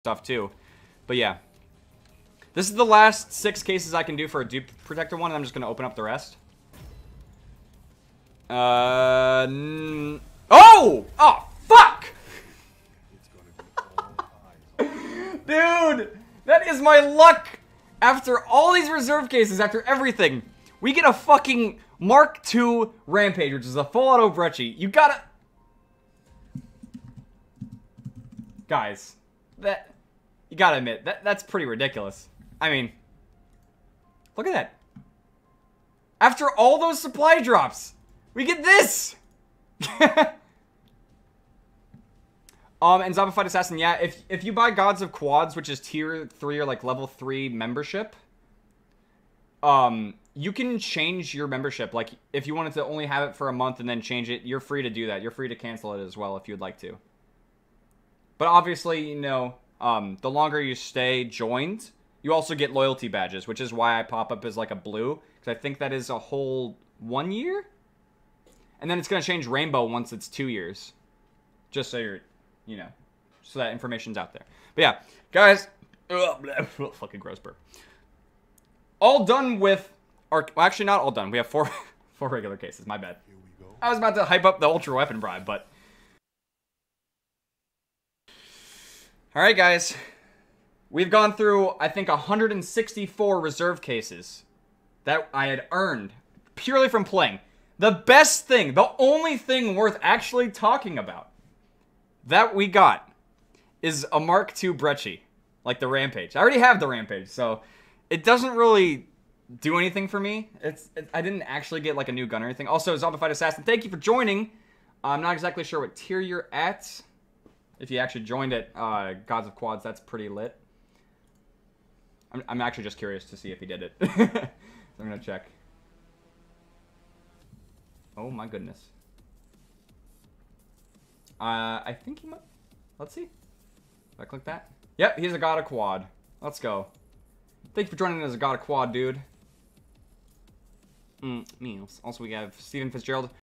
I'm Mentioned!!!!!!
So as of yesterday I bought the GODS OF QUADS as a membership from M3RKMUS1C and I wanted to be apart of the crew of course. Well it finally happened, furthermore he's been my favorite youtuber because he's never changed and he's always the same even when he switched everything up, I will always stay true as a loyal supporter of MERKMUS1C! I hope he sees this video.